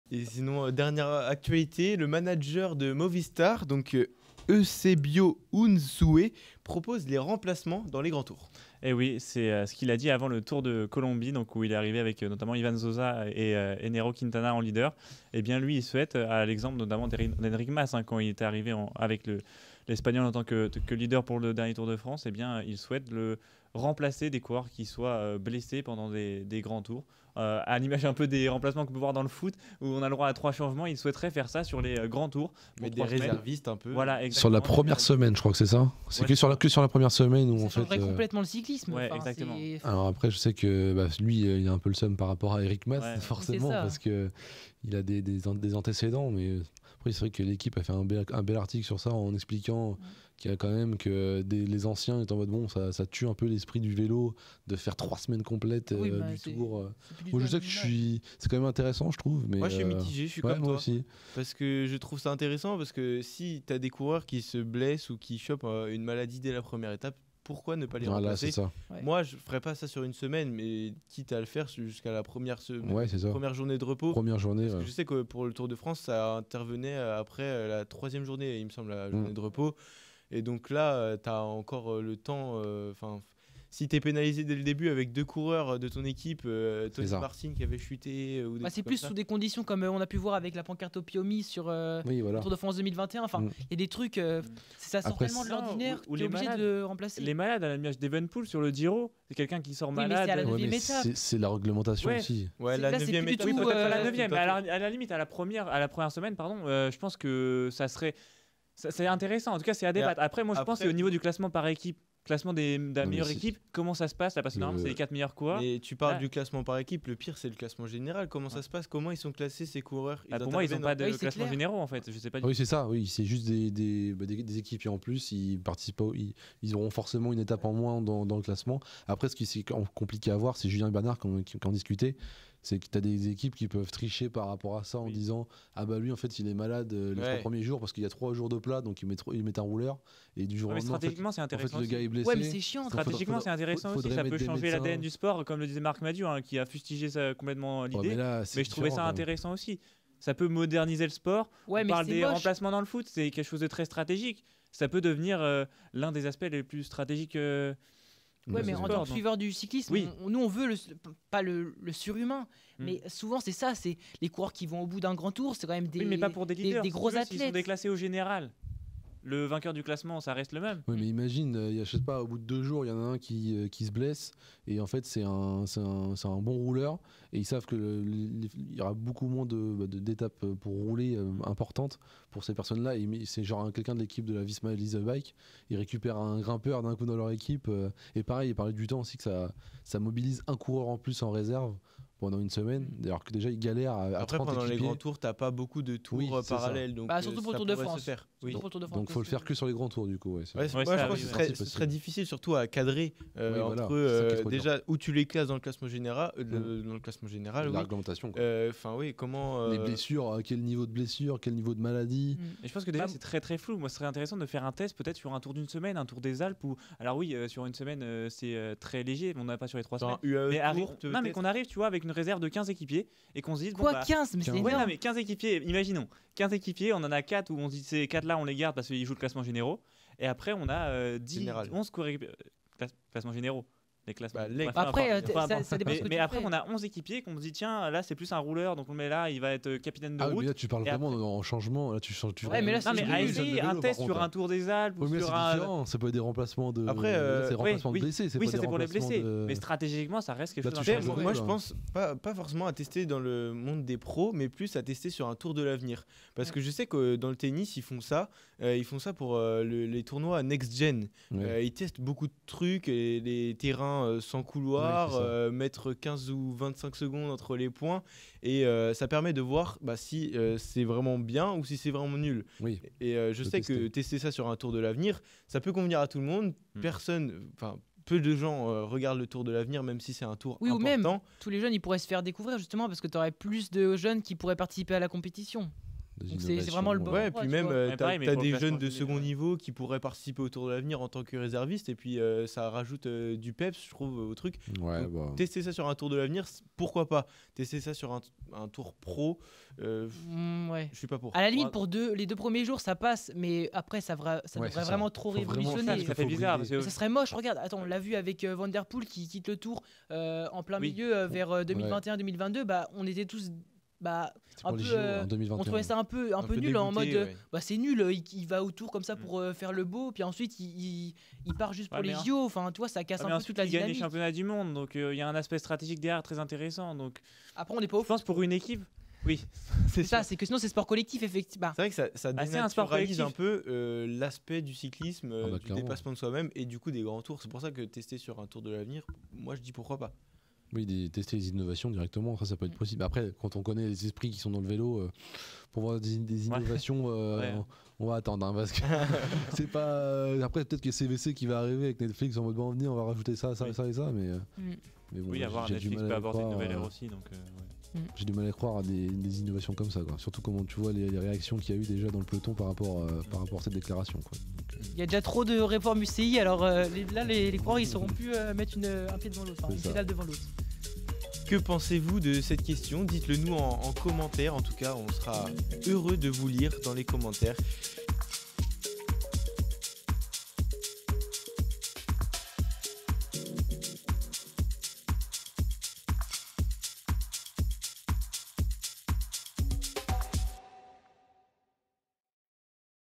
LE MANAGER DE MOVISTAR VEUT AUTORISER LES REMPLAÇANTS : UNE BONNE IDÉE ?
Eusebio Unzué (Movistar) a remis sur le feu l'idée des remplacements pendant les Grands Tours. Est-ce envisageable ? Ugo, Lilian, Tanguy & Titouan en ont discuté en ce début de semaine.
Bon visionnage !

Des posts quasi-quotidiens pour ceux qui veulent s'informer en 1 min !